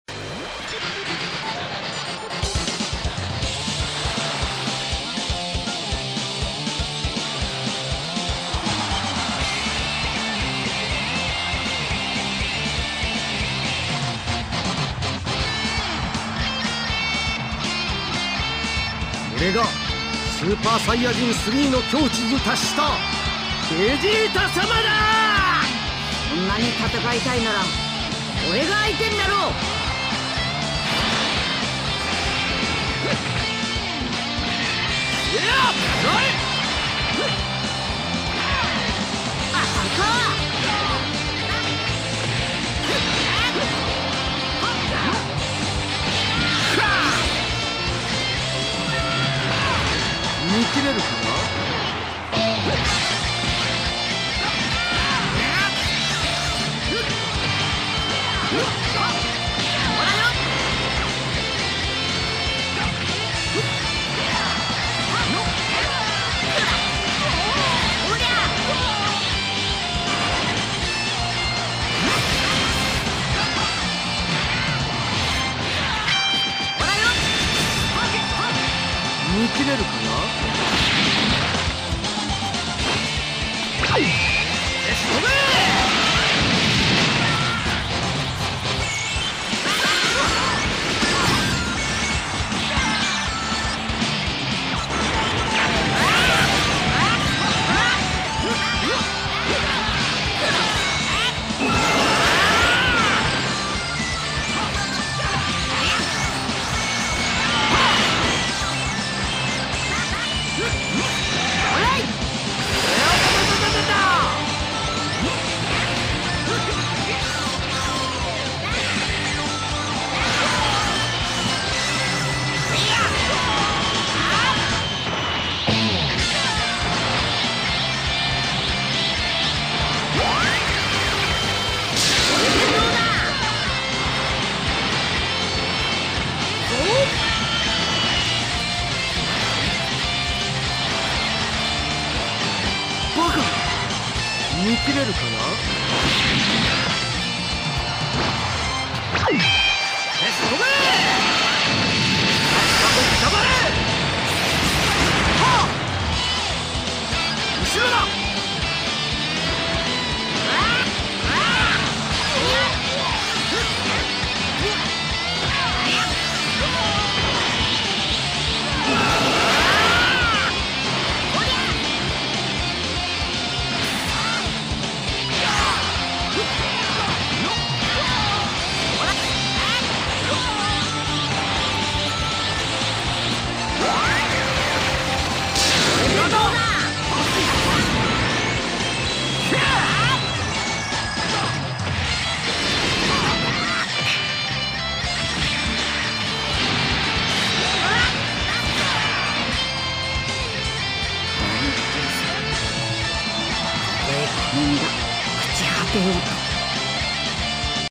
Cuartos de Final||Héctor vs Gery
Tercer combate de los cuartos de final, muy igualado la verdad!!!